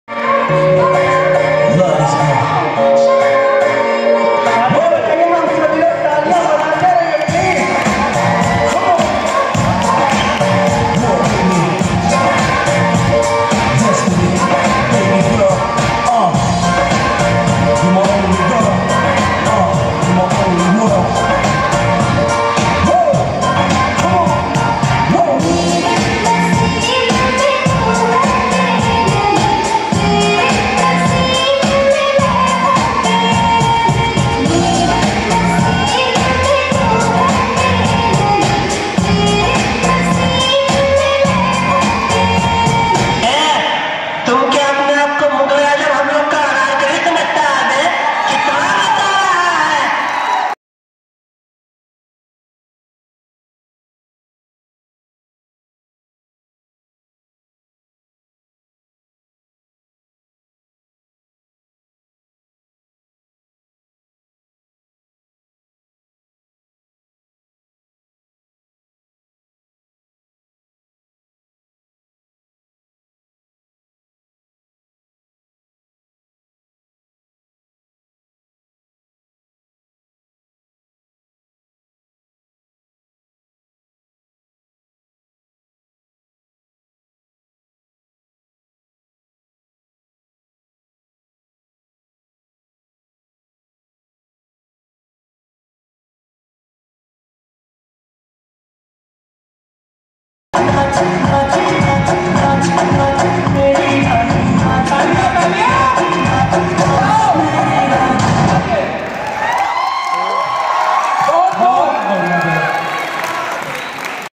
Seven year old girl dancing at her best at a talent fest | Chota packet bada dhamaka dance
This tiny cute little munchie girl dancing so well at talent fest.It is really eye soothing to watch her dance on the stage & make the audience gawk at her open mouth... Like how can She be so good and young.Girl is from Aryan dance planet.May god bless this child. Amen

We dont hold any claims over the songs used.It is only for entertainment.All copyrights are to their Original makers.